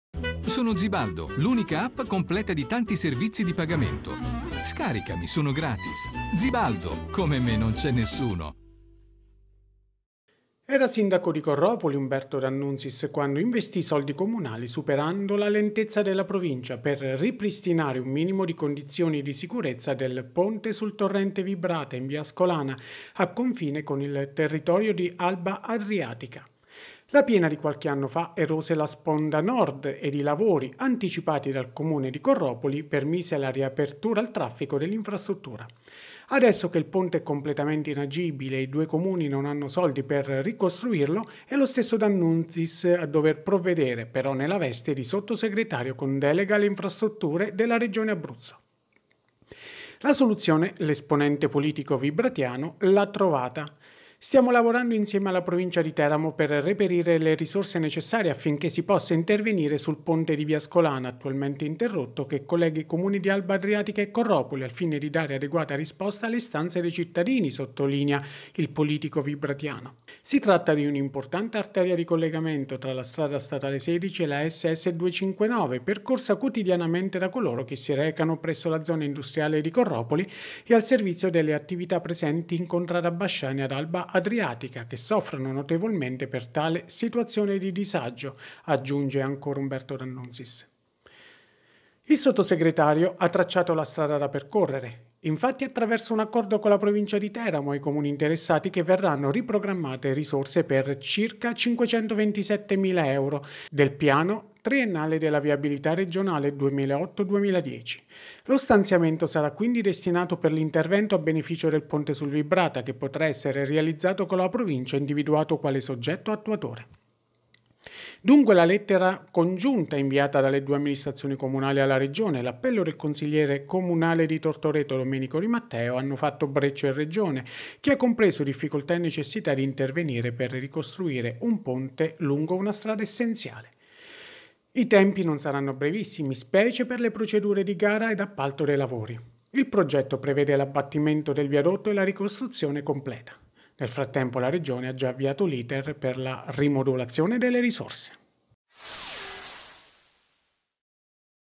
Alba Adriatica - Ponte sul Vibrata, la regione finanzierà la ricostruzione
Ne dà notizia il sottosegretario alla regione, Umberto d’Annuntiis che annuncia risorse per 527mila euro